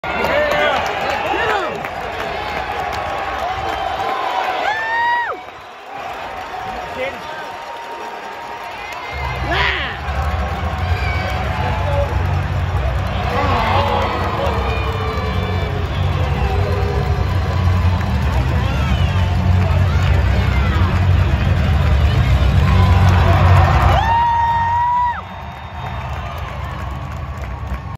Fight: Sean Allen Vs Christopher Cameron
Sean Allen of the Cincinnati Cyclones Vs Christopher Cameron of the Indy Fuel